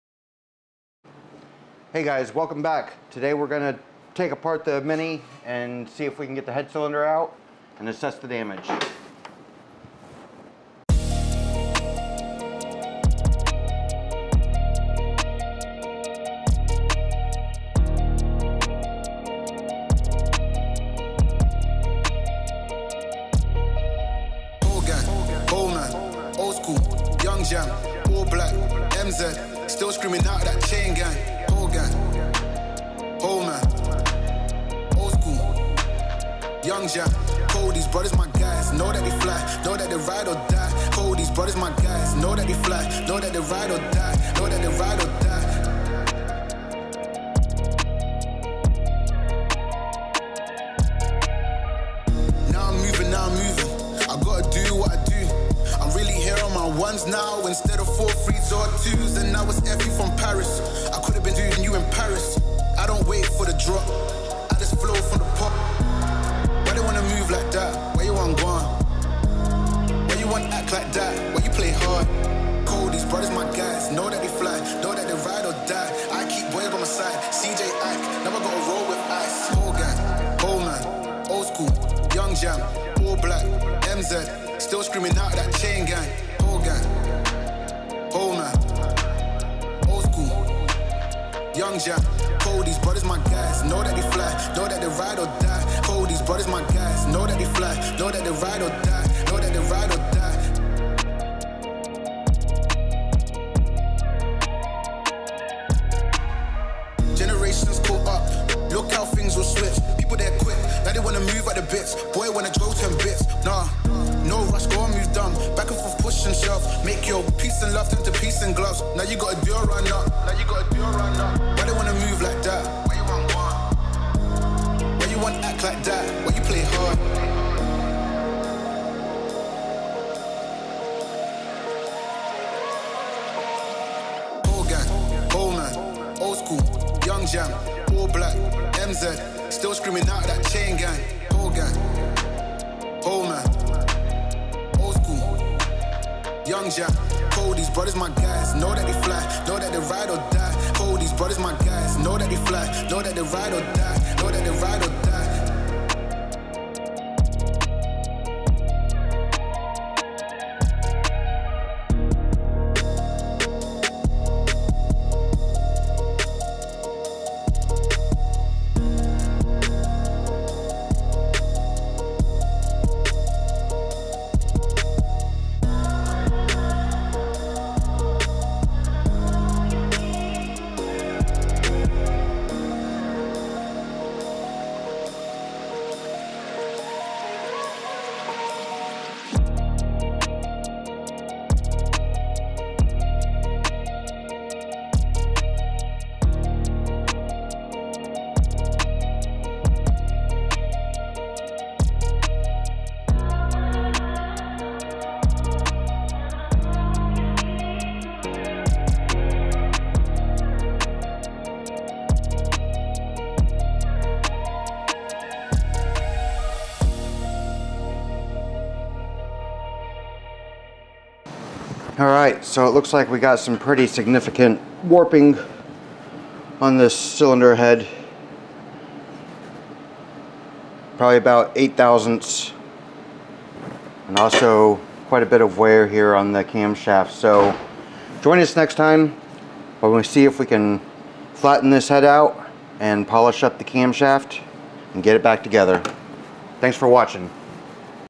Rescue Mini Cooper S R53 [Episode 2] Cylinder Head Removal
Help me keep the creative fires burning by buying me a coffee. Your support means the world to me. -

In Episode  2 we pull the mini cooper r53 s cylinder head and inspect the damage.